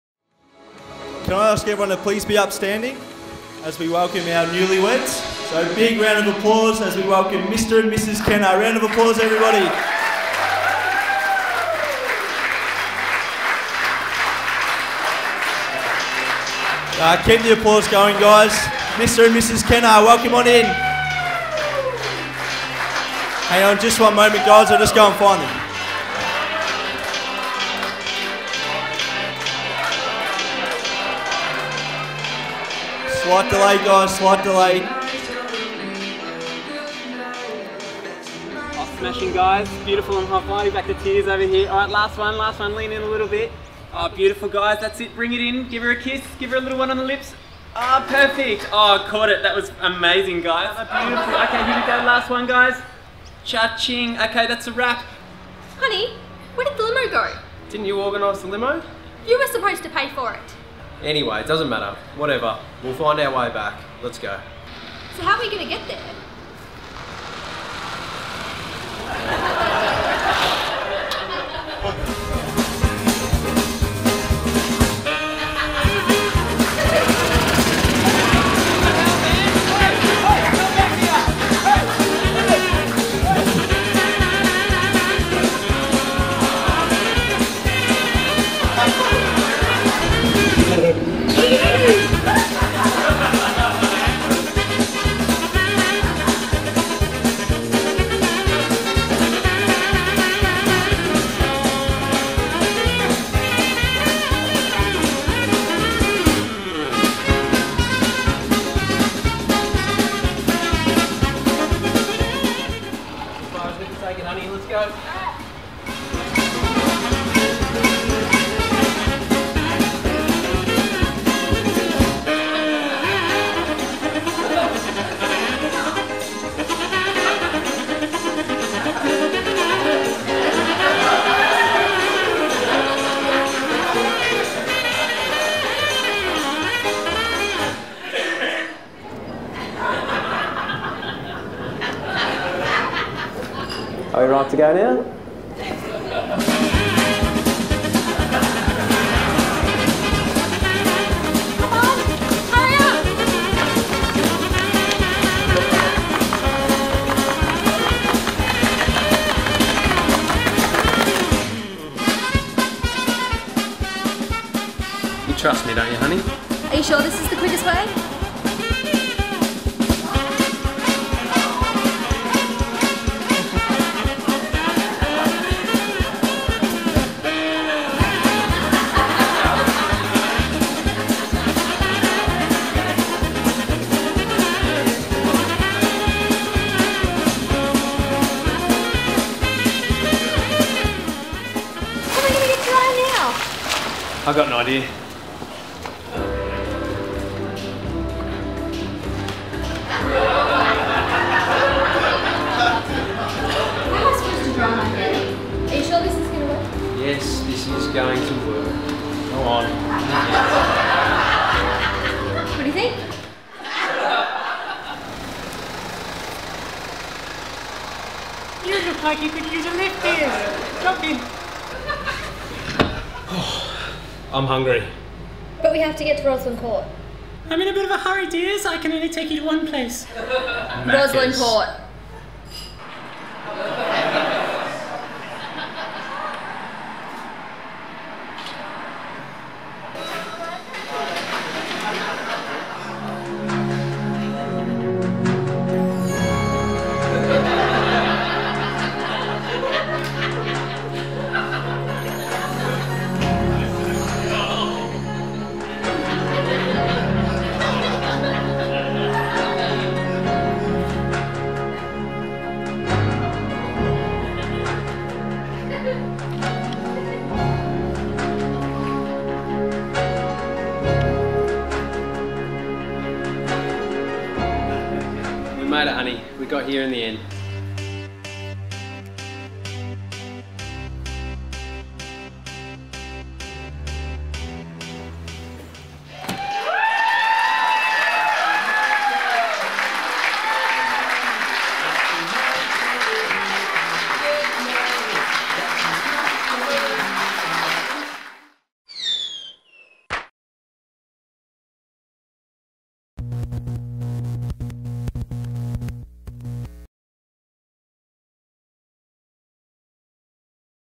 Funny Wedding Reception Entrance
We had some fun putting together a video for Taya & Pete for their wedding in Melbourne. The video show's what they went through to get from the Photo shoot to the reception in time.